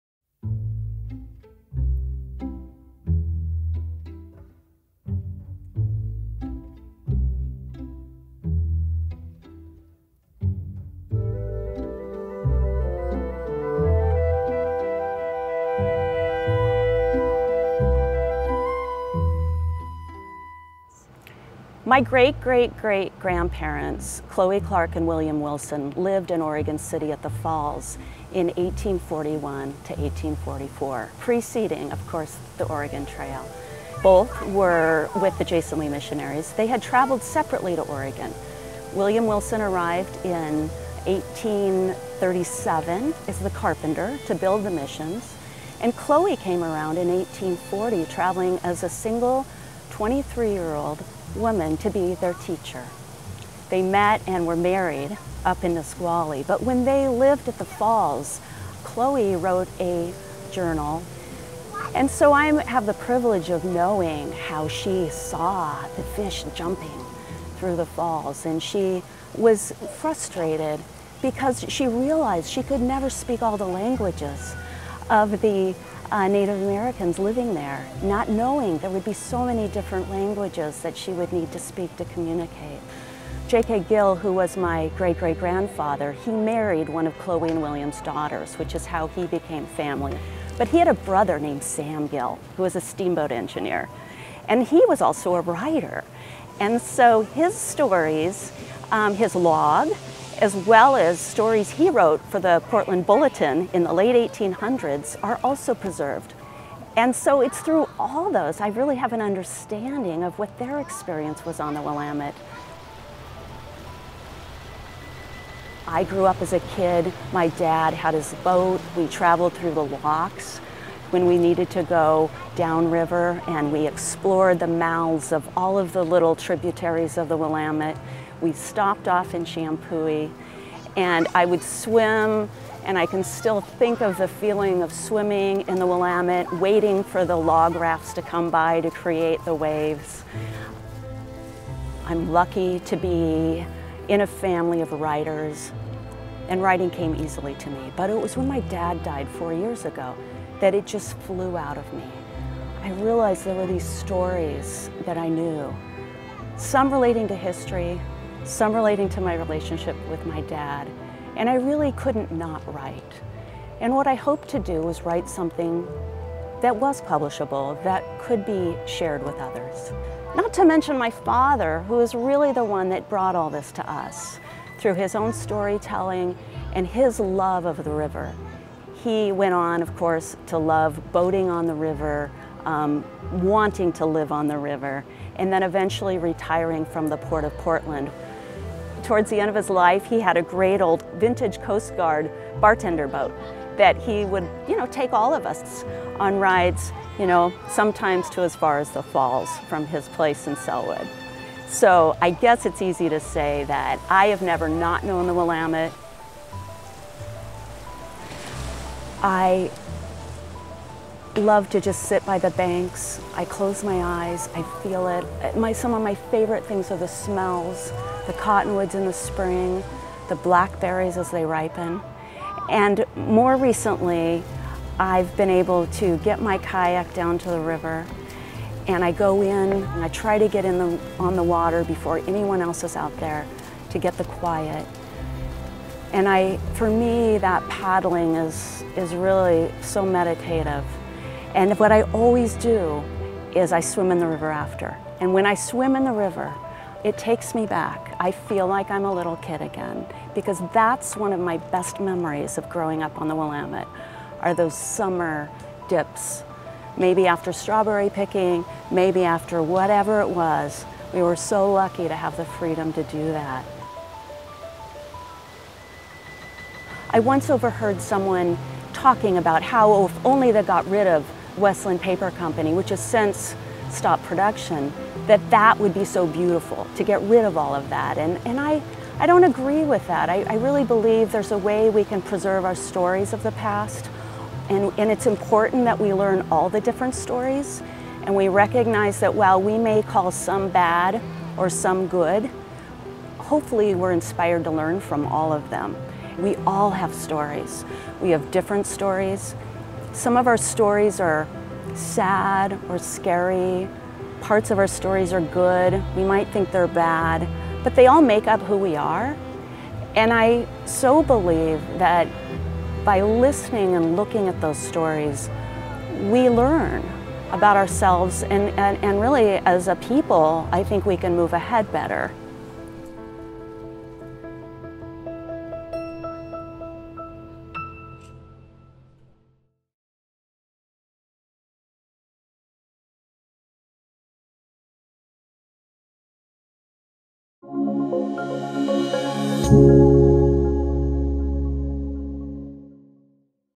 Stories of Willamette Falls: Dede Montgomery
Author Dede Montgomery shares the story of her families history with the Willamette River and Willamette Falls.